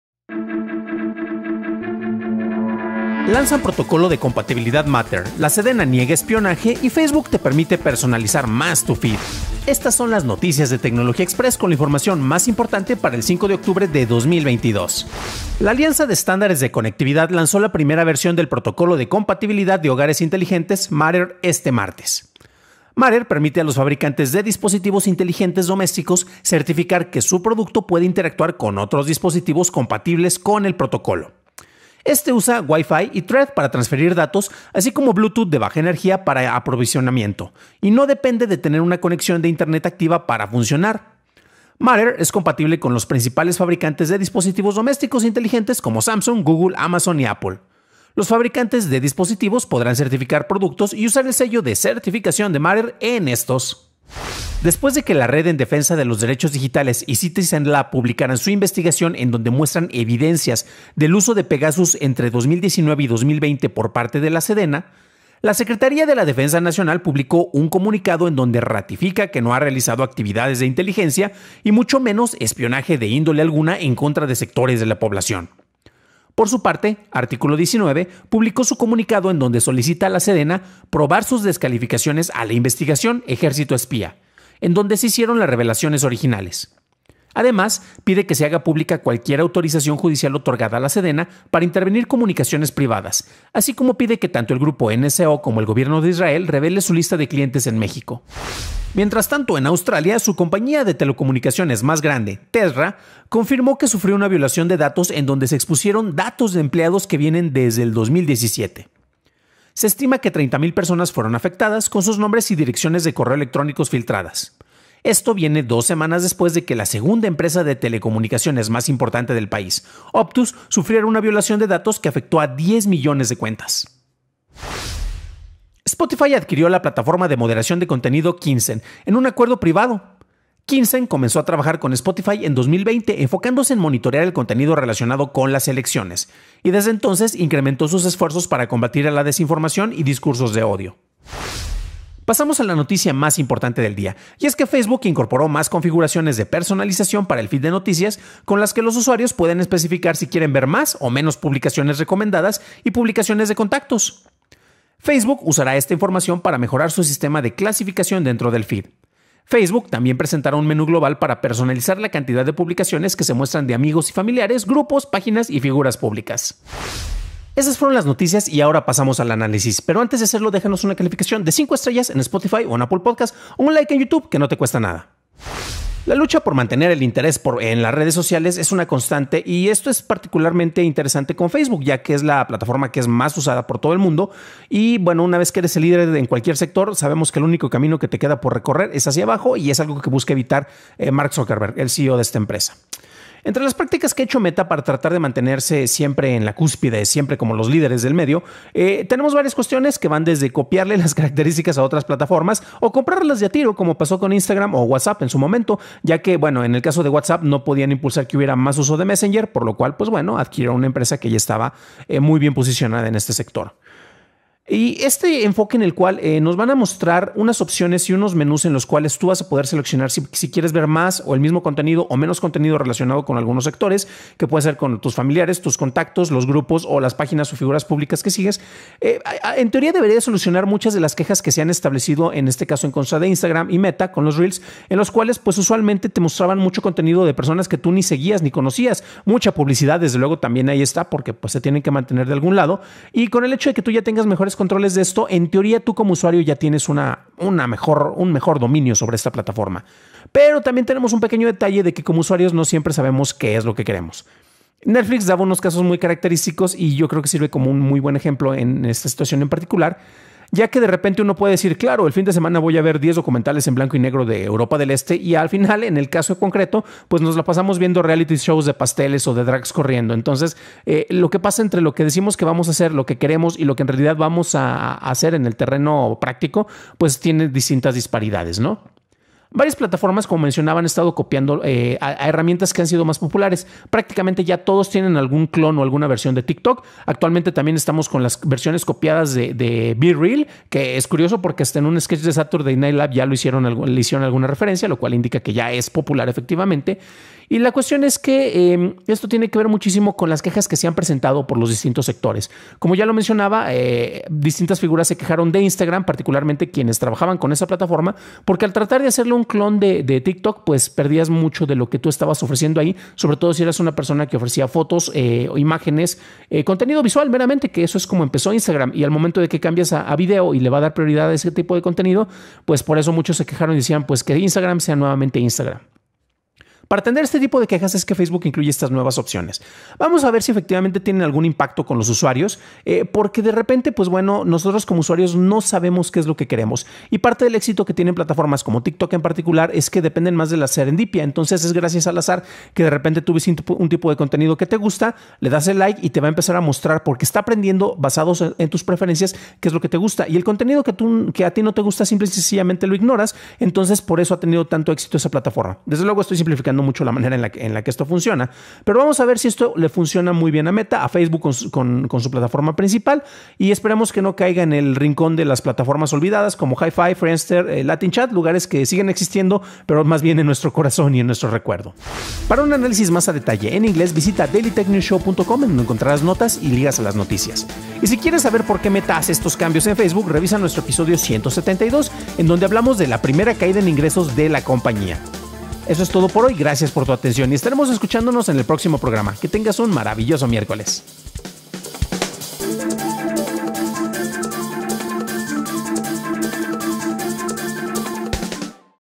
Facebook te ofrece más control – NTX 221
Noticias:

00:15 La Alianza de Estándares de Conectividad lanzó la primera versión del protocolo de compatibilidad de hogares inteligentes Matter este martes

00:54 La Secretaría de la Defensa Nacional publicó un comunicado en donde “ratifica que no ha realizado actividades de inteligencia y mucho menos espionaje de índole alguna en contra de sectores de la población”.

01:38 En Australia, su compañía de telecomunicaciones más grande, Telstra, confirmó que sufrió una violación de datos en donde se expusieron datos de empleados que vienen desde 2017

02:04 Spotify adquirió la plataforma de moderación de contenido Kinzen, en un acuerdo privado.

02:21 Facebook incorporó más configuraciones de personalización para el feed de noticias, con lo que los usuarios pueden especificar si quieren ver más o menos publicaciones recomendadas y publicaciones de contactos.

02:55 Análisis: ¿Qué contenido buscamos?